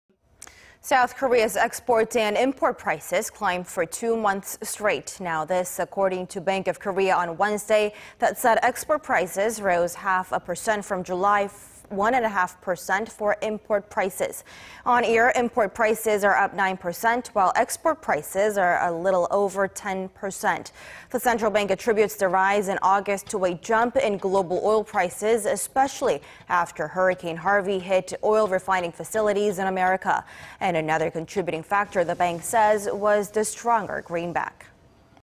Import and export prices increase for second month in a row
8월 수출입물가 두 달 연속 상승…국제유가 영향

  South Korea's export and import prices climbed for 2 months straight.
  This, according to Bank of Korea Wednesday,... that said export prices rose half a percent from July, 1-and-a-half for import prices.
On-year, import prices are up 9 percent... export prices a little over 10 percent.
  The central bank attributes the rise in August to a jump in global oil prices, especially after Hurricane Harvey hit oil-refining facilities in America.
Another contributing factor was the stronger greenback.